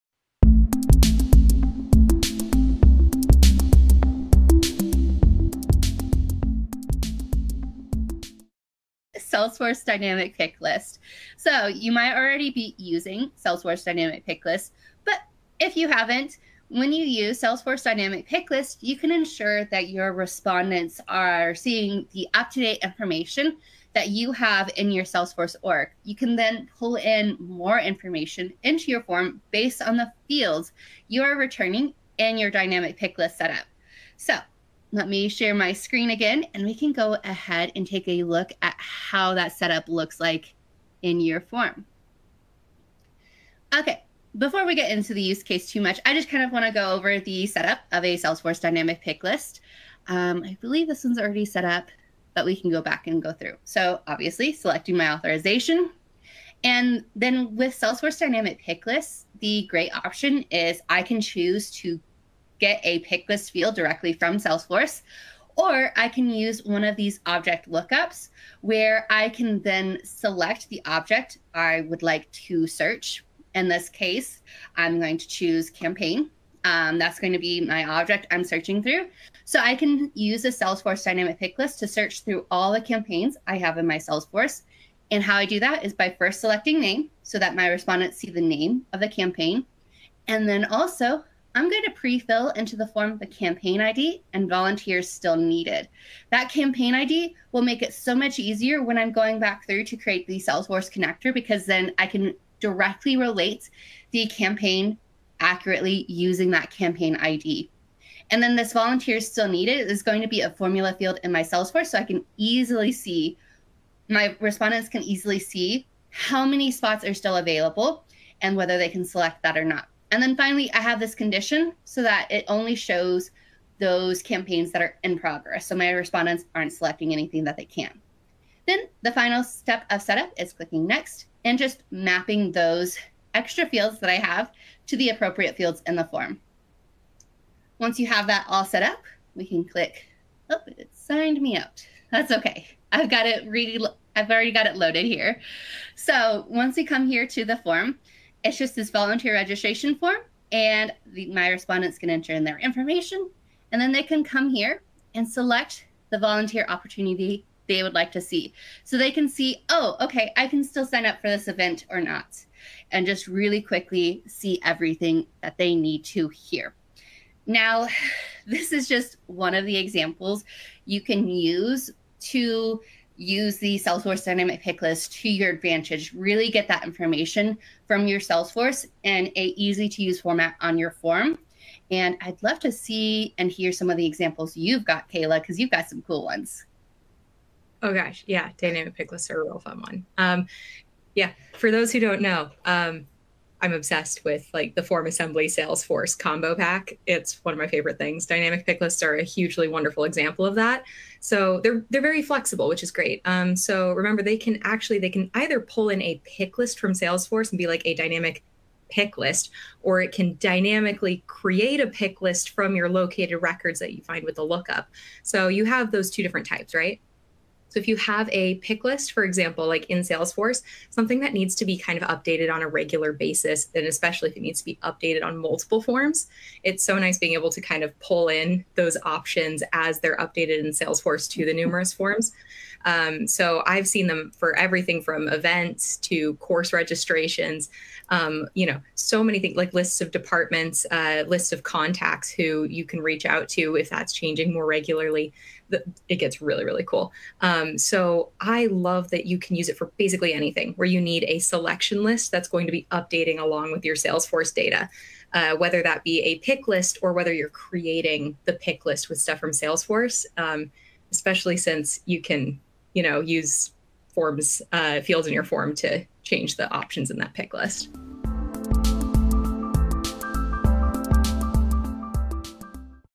How to Add Salesforce Dynamic Picklist to a Web Form - User Conference Clip
Learn how to improve your forms with dynamic picklists sourced directly from your Salesforce CRM. In this tutorial, we'll show you how to connect a Salesforce dynamic picklist directly to your form. By connecting to dynamic picklist values in Salesforce, you'll provide responsive options for form respondents while reducing CRM data cleanup on the backend. Watch now to improve your form interactions and data quality